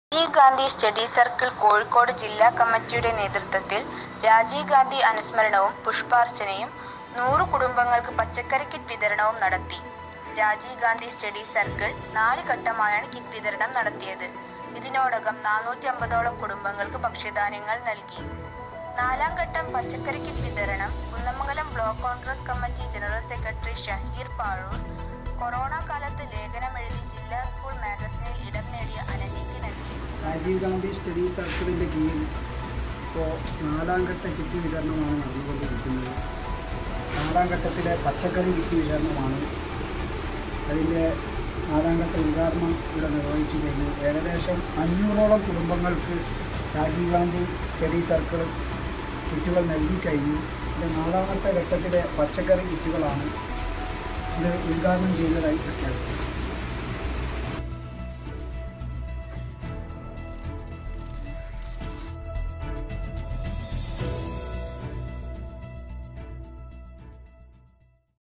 രാജീവ്‌ ഗാന്ധി സ്റ്റഡി സർക്കിൾ കോഴിക്കോട് രാജീവ്‌ ഗാന്ധി അനുസ്മരണവും ഭക്ഷണ കിറ്റ് വിതരണം നടത്തി
Mavoor news media spot